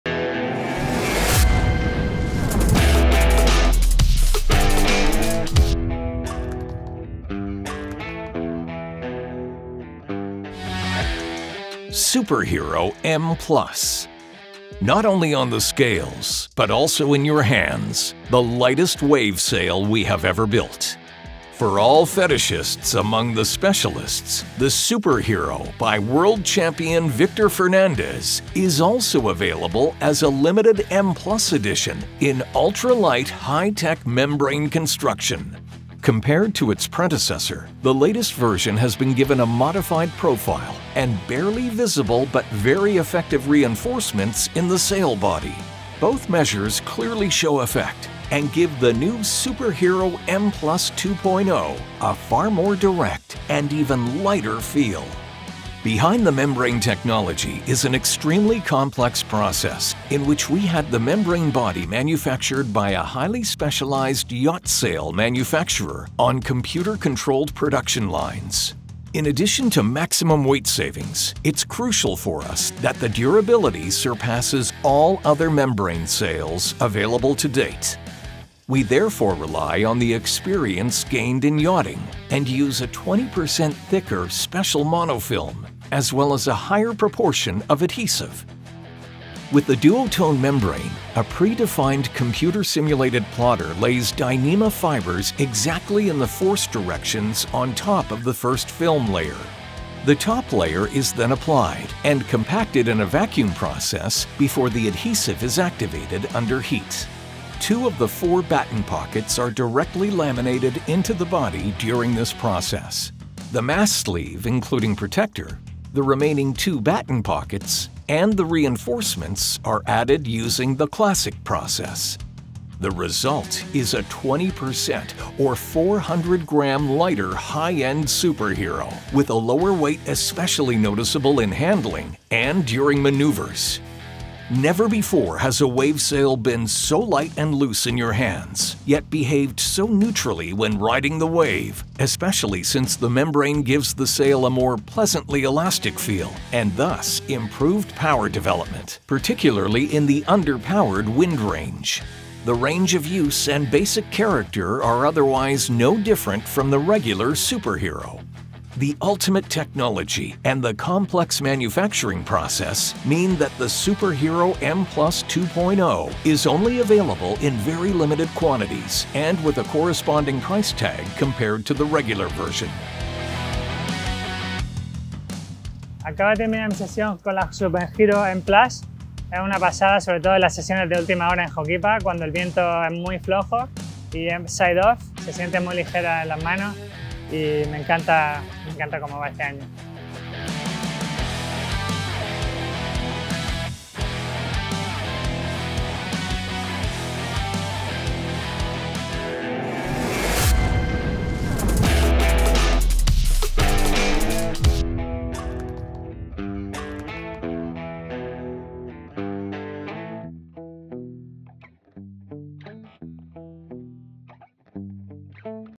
SUPER_HERO M+ 2022 Product Clip | Duotone Windsurfing
WAVE ULTRALIGHT - NOT ONLY PHYSICALLY BUT ALSO IN YOUR HANDS THE LIGHTEST WAVE SAIL WE HAVE EVER BUILT! For all fetishists among the specialists, the SUPER_HERO by World Champion Victor Fernandez is also available as a limited M+ edition in ultra-light high-tech membrane construction. Compared to its predecessor, the latest version has been given a modified profile and barely visible but very effective reinforcements in the sail body. Both measures clearly show effect and give the new SUPER_HERO M+ 2.0 a far more direct and even lighter feel!
Behind the membrane technology is an extremely complex process in which we have the membrane body manufactured by a highly specialized yacht sail manufacturer on computer-controlled production lines. In addition to maximum weight savings, it is crucial for us that the durability surpasses all other membrane sails available to date. We therefore rely on the experience gained in yachting and use a 20% thicker, special monofilm as well as a higher proportion of adhesive.
With the DUOTONE membrane, a predefined computer simulated plotter lays Dyneema fibres exactly in the force directions on top of the first film layer. The top layer is then applied and compacted in a vacuum process before the adhesive is activated under heat. Two of the four batten pockets are directly laminated into the body during this process. The mast sleeve, including protector, the remaining two batten pockets and the reinforcements are added using the classic process. The result is a 20% (= 400 g) lighter high-end SUPER_HERO, whose lower weight is especially noticeable during manoeuvres and handling. Never before has a wave sail been so light and loose in your hands yet behaved so neutrally when riding the wave, especially since the membrane gives the sail a more pleasantly elastic feel and thus improved power development, particularly in the underpowered wind range. The range of use and basic character are otherwise no different from the regular SUPER_HERO.
The ultimate technology and the complex manufacturing process mean that the SUPER_HERO M+ 2.0 is only available in very limited quantities and at a price significantly higher than the regular version.